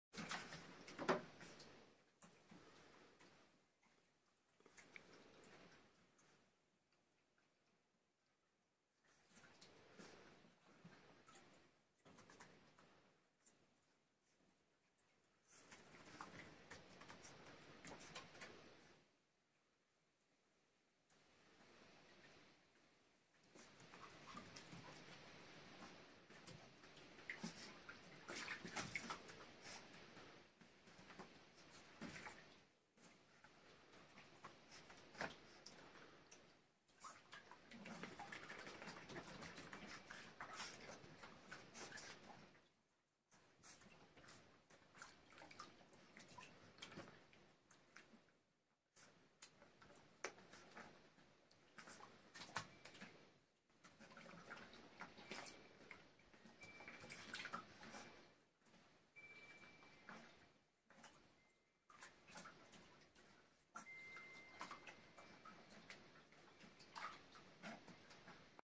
Shih Tzu Pool Party
Tahlia and Taelyn love the pool, Tzuriel isn't so keen though, he stayed inside out the way.